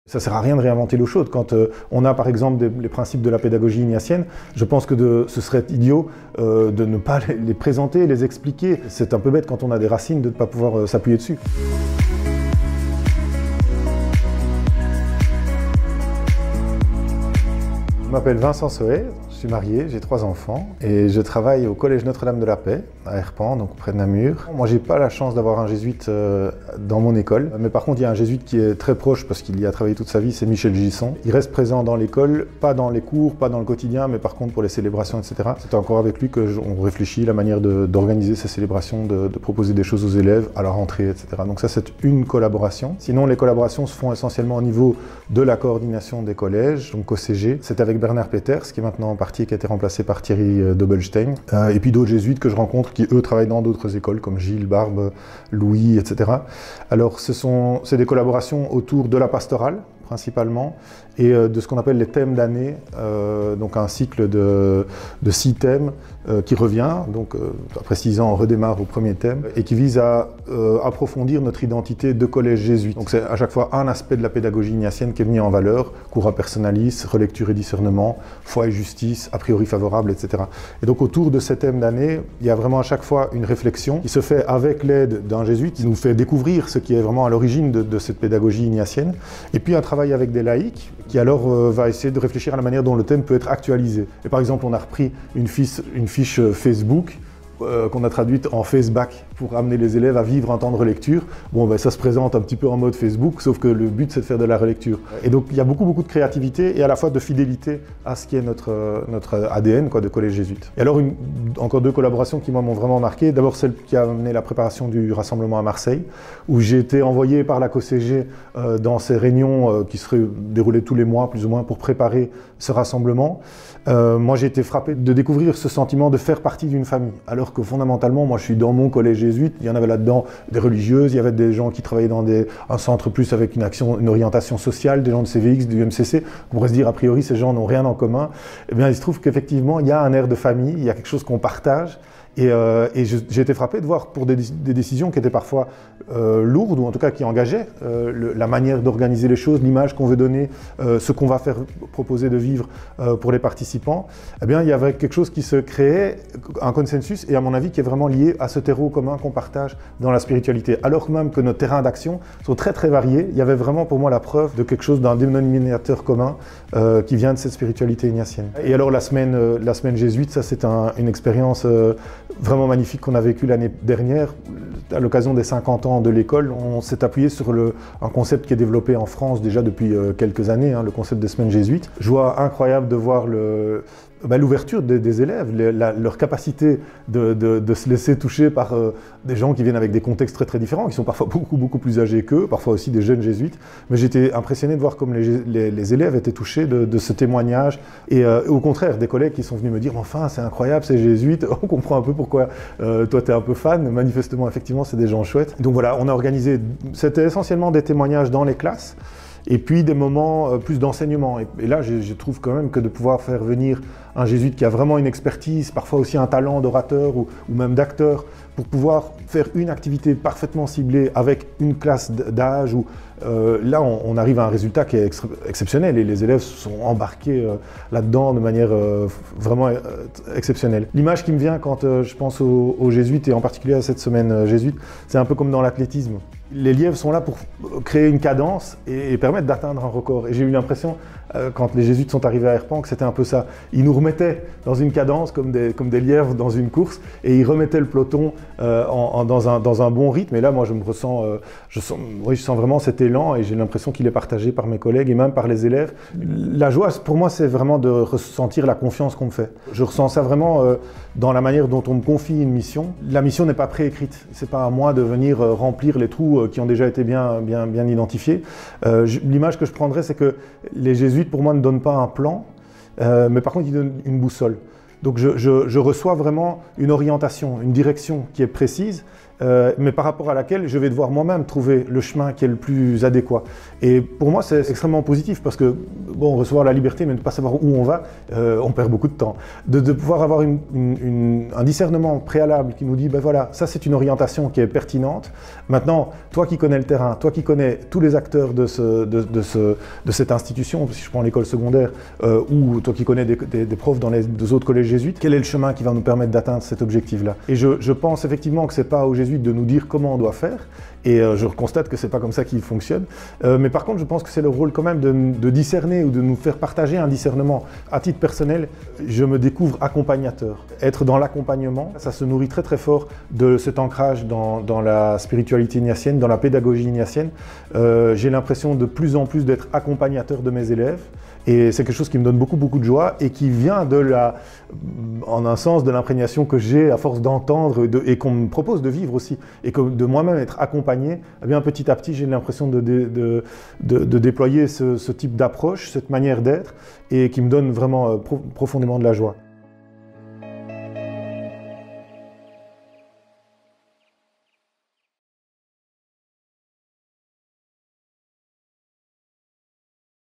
Collaboration entre jésuites et laïcs - Interview de Vincent Sohet
Du 27 au 30 décembre 2022 s’est déroulée l’assemblée annuelle de la Province, au Lycée Sainte-Geneviève (Ginette), à Versailles. Réunissant 130 jésuites et 50 laïcs, elle était consacrée à la manière de vivre la collaboration ensemble. Nous retrouvons aujourd'hui Vincent Sohet, professeur au Collège Notre-Dame de la Paix à Erpent. Il nous parle de sa collaboration avec des jésuites Belges au sein de l'établissement scolaire.